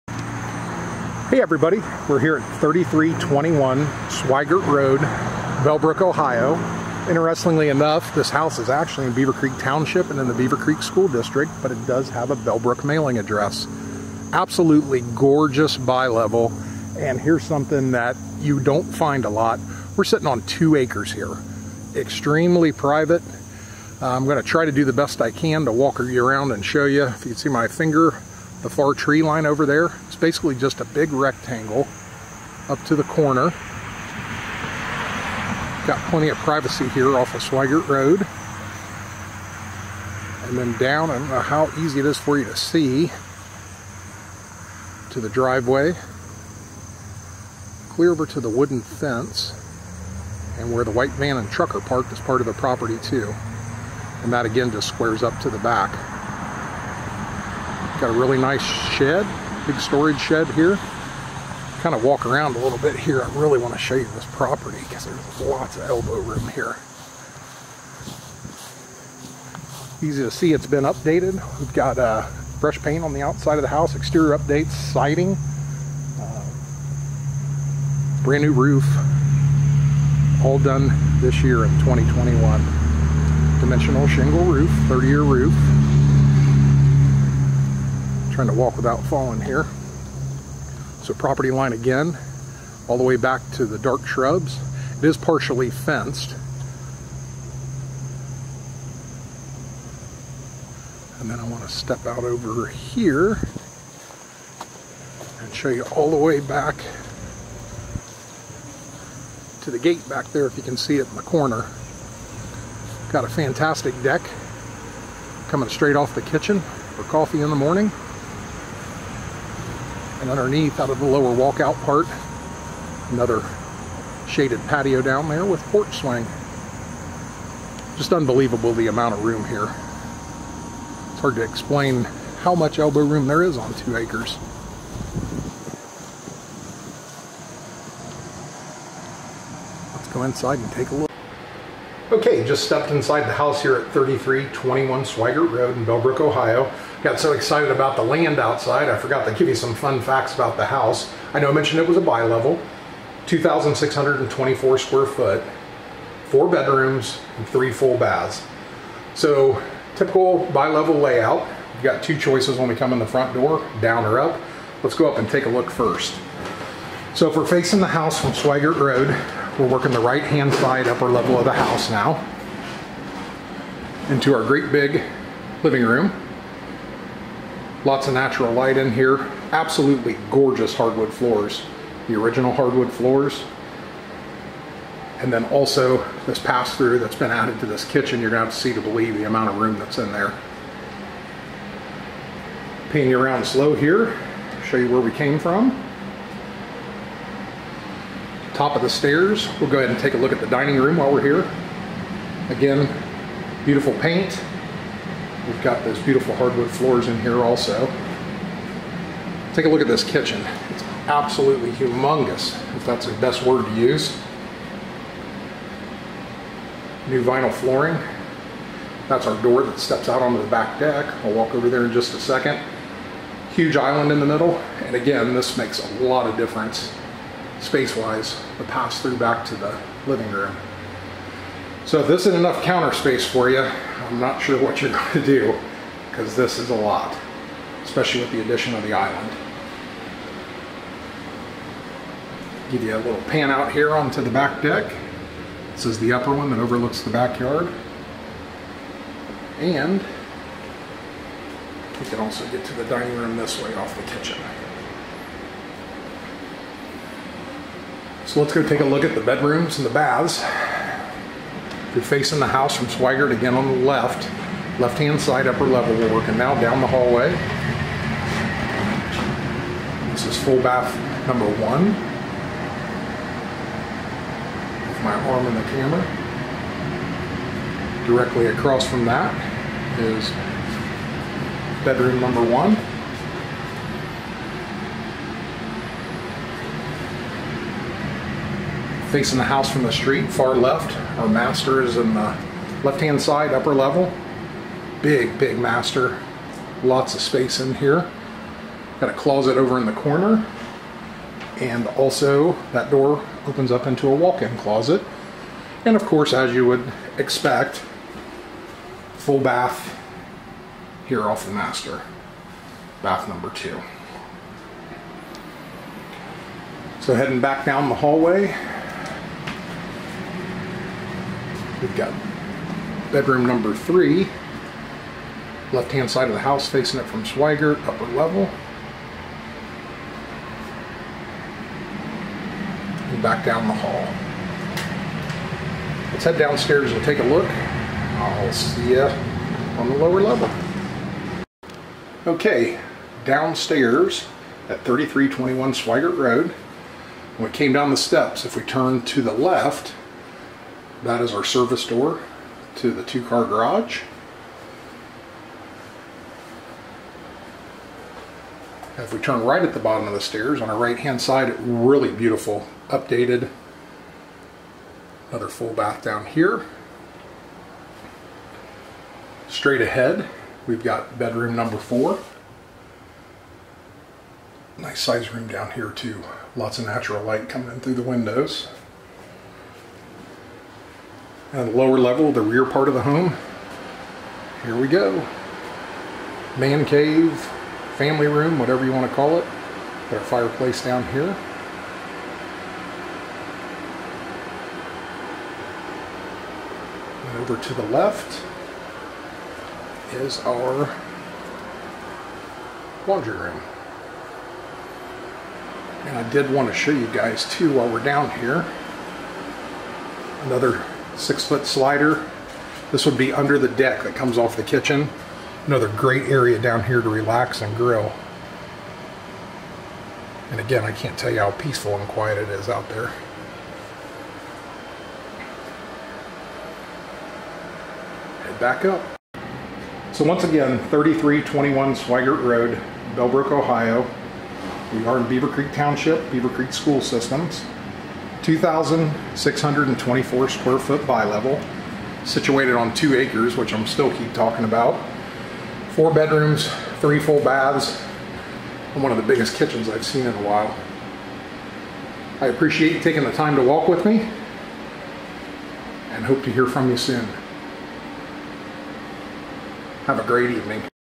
SOLD! 3321 Swigart Rd, Bellbrook, OH 45305 ~ Pride of ownership shows throughout over 2,600 Sq Ft!
3321 Swigart Rd, Bellbrook, OH 45305 ~ $294,900
Want country living while being close to everything...then look no further. This 2,624 sq ft 4 bedroom 3 full bath bi-level is the one. Situated on 2 private acres. As soon as you pull in you will know this home has been loved. New exterior paint, vinyl siding and dimensional shingle roof all new in 2021. Step inside and things get even better. Upper level boasts a large living room, dining room, the spacious master with walk in closet and dedicated full bath, along with 2 additional bedrooms and an updated full bath. The kitchen is huge and a must see. Counter space galore with even more room at the island. Step out of the kitchen and enjoy the second story deck while you are there. Lower level is absolutely fantastic as well. Large family room, gorgeous full bath, access to the 2 car garage and bedroom number 4. Don't miss the patio area off the family room! AC unit: 2018. Water Heater:2012. Schedule a showing today!
Welcome to Bellbrook, OH
Welcome to Beavercreek Township, OH
Welcome to Beavercreek Schools
Beavercreek Elementary School
Beavercreek Middle School
Beavercreek High School